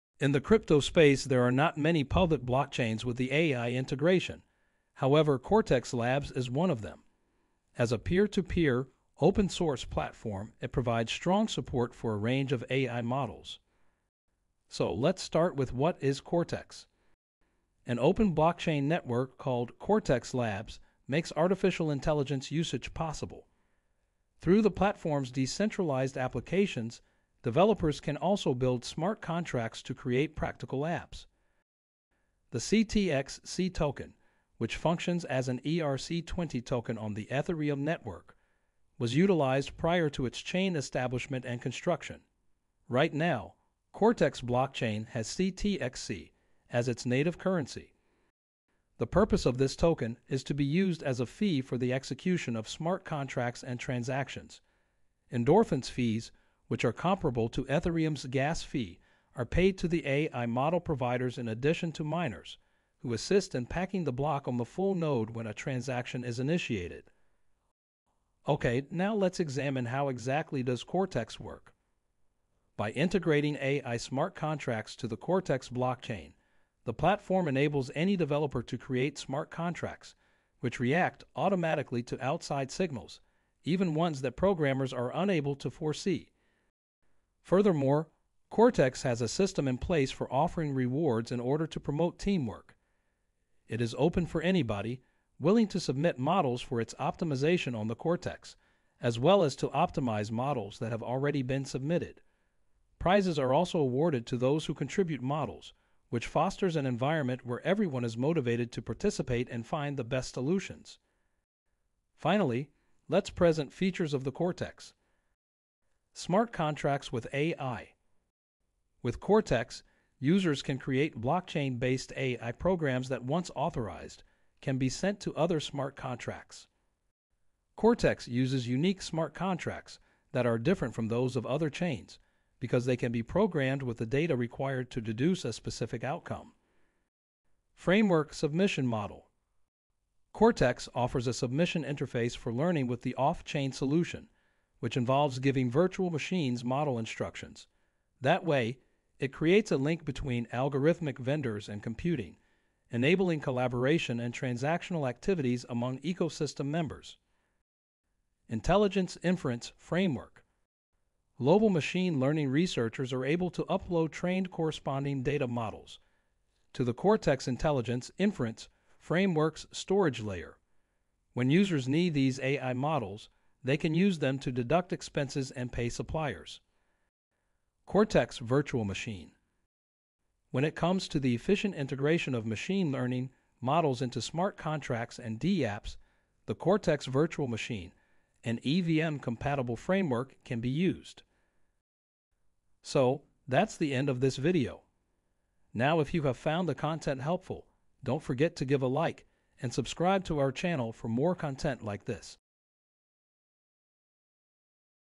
What is Cortex? - Cortex CTXC Machine Learning Blockchain Explained
This video answers the questions: What is Cortex? What is Cortex CTXC Cryptocurrency? How does Cortex work? - Cortex CTXC Machine Learning and AI Blockchain will be explained, reviewed, and presented.

Timestamps:
0:00 Introduction
0:15 What is Cortex?
1:09 How exactly does Cortex work?
1:53 Features of the Cortex
3:12 Video end

AI models can be uploaded and run across a distributed network using the open-source, peer-to-peer, decentralized Cortex (CTXC) blockchain platform, which is built on Ethereum. The goal of the Cortex platform is to make AI more accessible by making it simple to construct decentralized applications (DApps) and incorporate models into smart contracts. Similar to standard contracts, smart contracts are implemented as blockchain protocols rather than being written on paper.

Until the Cortex team, there was no known method for effectively and deterministically facilitating on-chain machine learning models. Consequently, the goal of smart contracts was undermined by DApps using off-chain conclusions, including machine learning models. The goal of Corex was to fix the issue with the Cortex Virtual Machine, which ran on the GPU instead of the CPU. On the Cortex blockchain, the Cortex Virtual Machine (CVM) aims to enable effective machine learning. The platform makes it possible to incorporate machine learning models into decentralized apps and smart contracts.  Furthermore, developers employ off-the-shelf AI models and the well-known language Solidity to create AI-enhanced DApps and smart contracts.

The whitepaper states that Cortex's ultimate objective is to improve as a token that works with all AI models. In order to accomplish this, Cortex 2.0 aims to provide enhanced security and performance for the Cortex chain as well as a more extensive core technological architecture enabling on-chain AI integration in smart contracts. The Cortex 2.0 implementation also uses trusted execution environments, formal verification, and other techniques to highlight the privacy and accuracy of on-chain AI inference outputs.

Not only in this video various crypto topics will be explained.